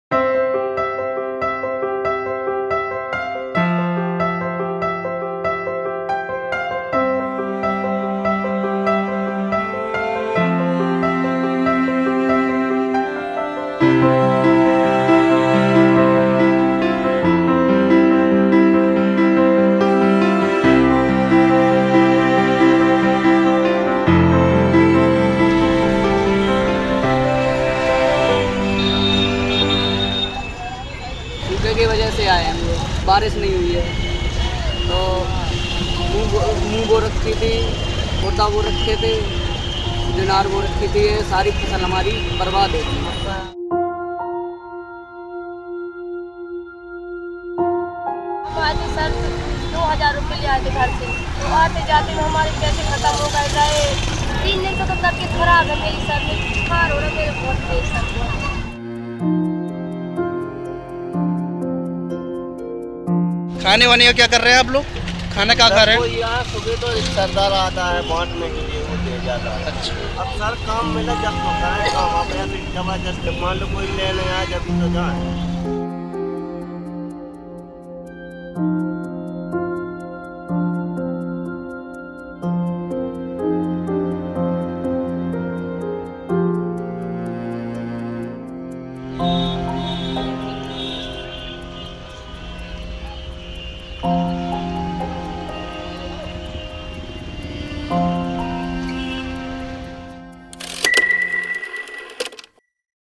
A Day Spent with Delhi's Migrant Labours
Watch this video to know if how Migrant labours spent their first day in Delhi - the city of opportunities.